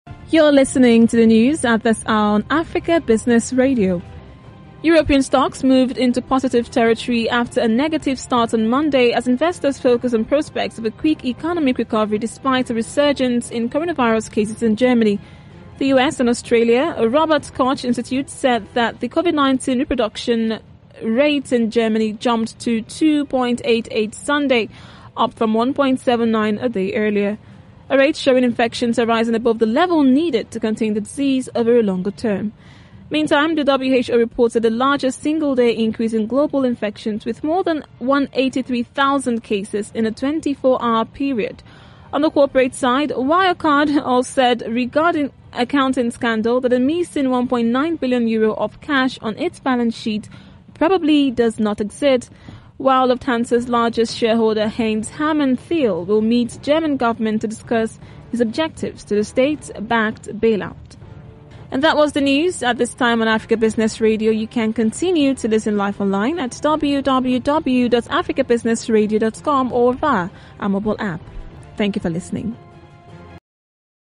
European Stocks Turns Positive.
European stocks moved into positive territory after a negative start on Monday, as investors focus on prospects of a quick economic recovery despite a resurgence in coronavirus cases in Germany, the US and Australia. Robert Koch Institute said that the covid-19 reproduction rate in Germany jumped to 2.88 Sunday, up from 1.79 a day earlier, a rate showing infections are rising above the level needed to contain the disease over the longer term. Meantime, the WHO reported the largest single-day increase in global infections, with more than 183 thousand cases in a 24-hour period.